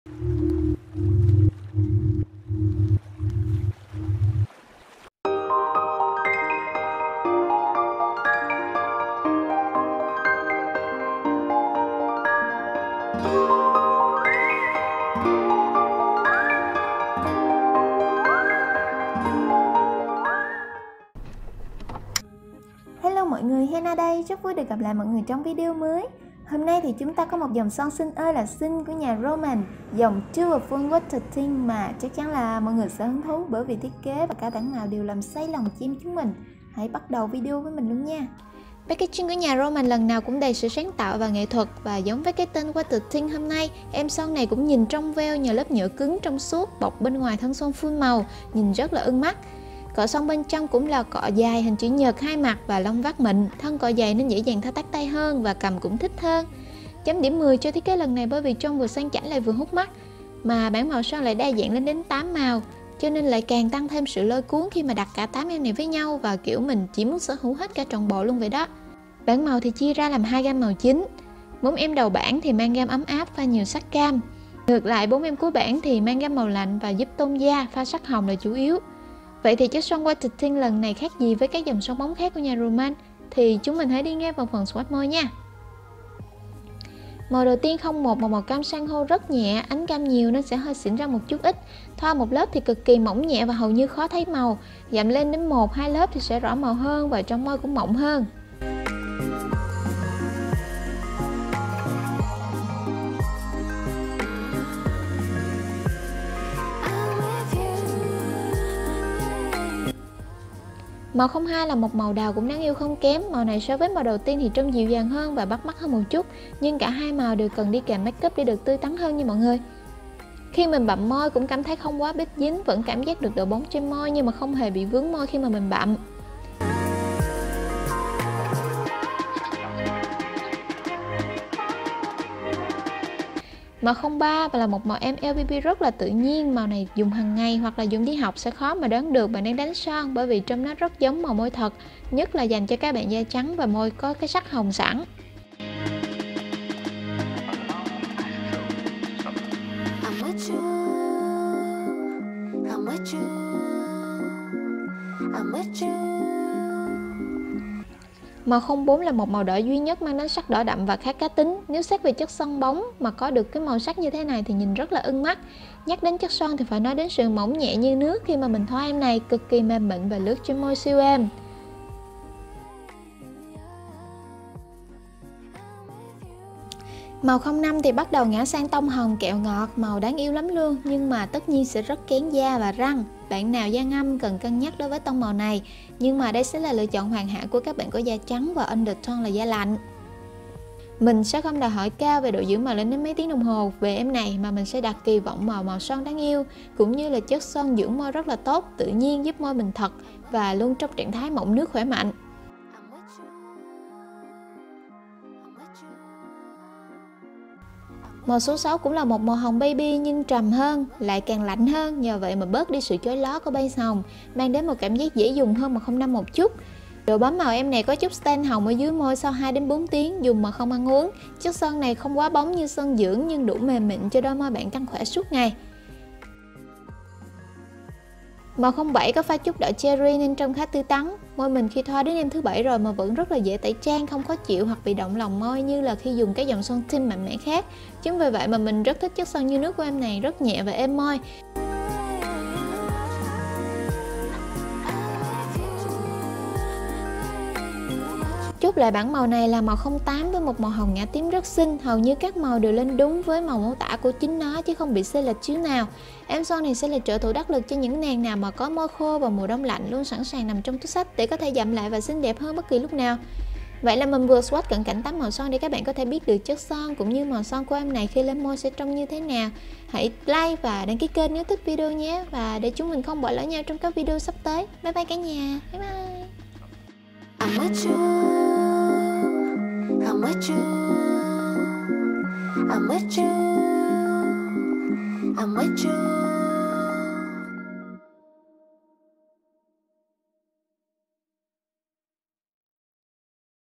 U là tời son đẹp quá 🍬Swatch & Review full 8 màu son ROMAND DEWYFUL WATER TINT 🌸
Hi mọi người, cẩn thận kẻo choảng trước vẻ đẹp của dòng son mới này nha~~~

© Bản quyền thuộc về Hena Makeup
© Copyright by Hena Makeup ☞ Do not Reup